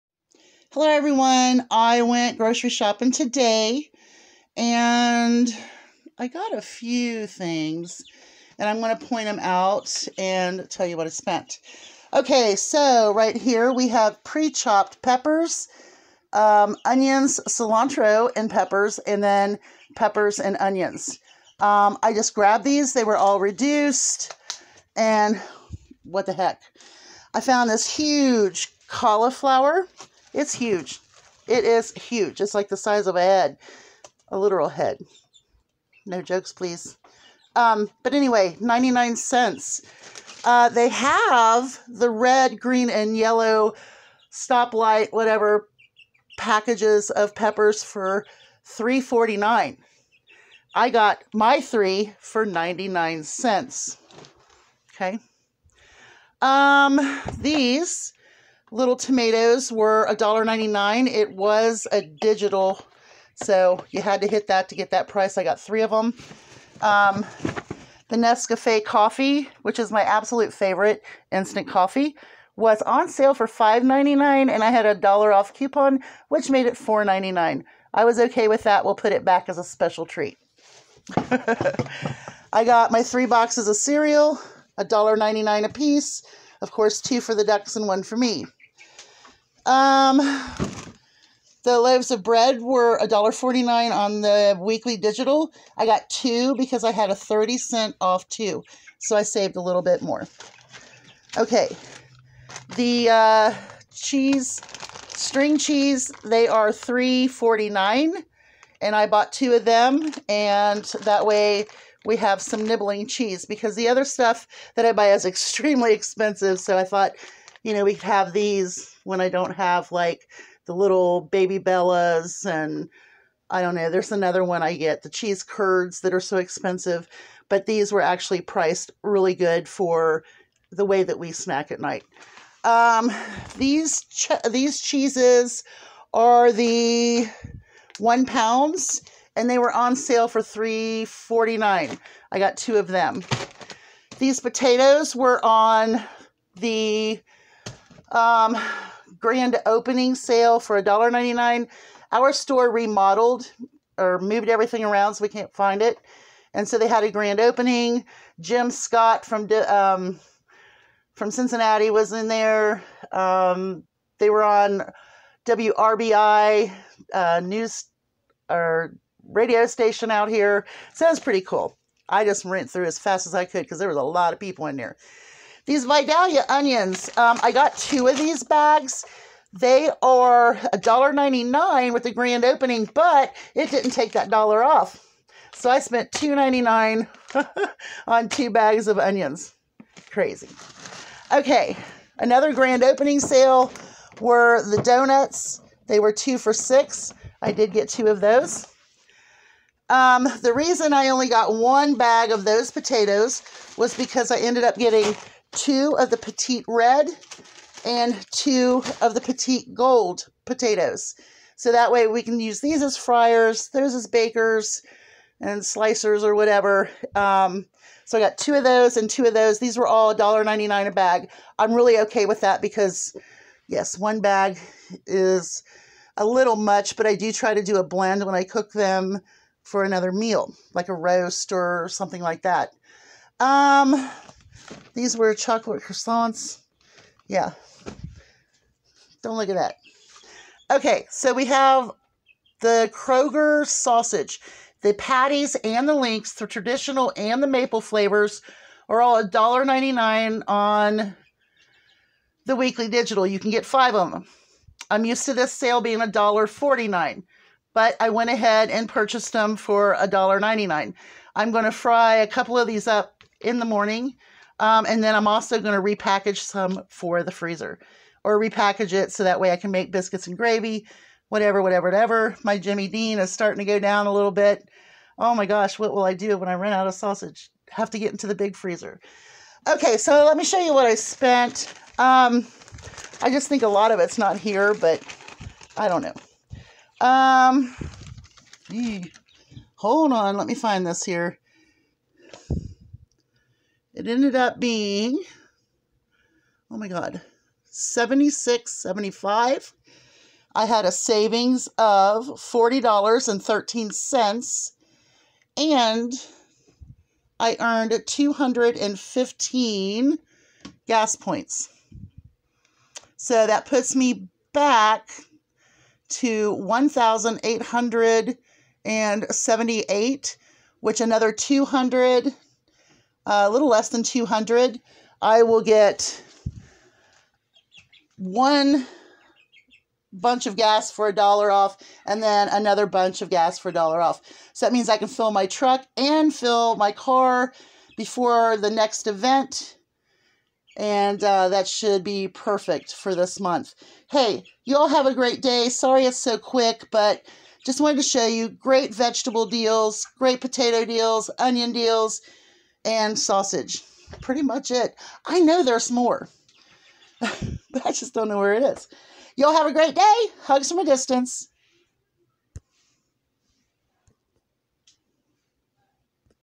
Kroger 4X fuel point and grand opening event deals and sales, haul!
a quick show of most of what I got today at kroger. reduced vegetables, grand opening sales, weekly digitals and digital coupons. I think it is funny that they are calling it a grand opening at our Kroger when all they have done was just move things around changed the aisle with and bought new shopping carts. anyway good deals good sales check it out.